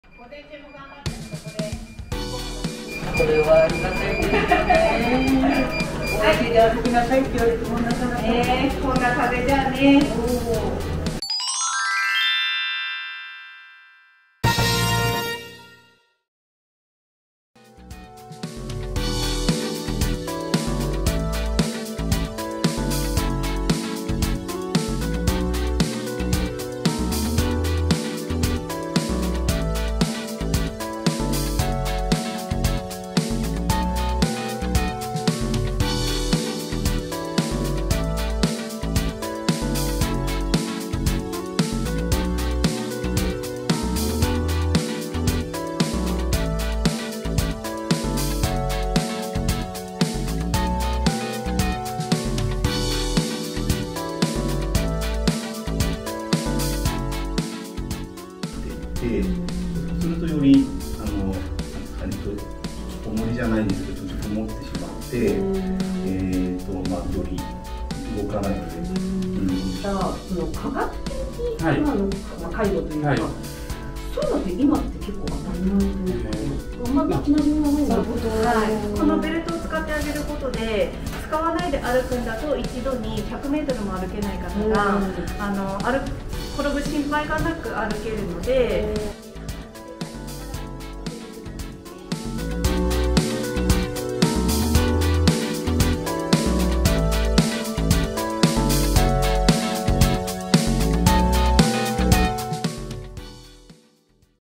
りふれ中野林デイサービスセンターが取材を受けました！
（公財）JKA様のwebサイト「cycle」取材班が、今期補助事業先となりました りふれ中野林 に来所。
３連Ｐウォーク について、ご利用者様と、理事長、担当職員へインタビュー取材されました。
当日の様子を動画にしましたのでぜひご覧くださいね！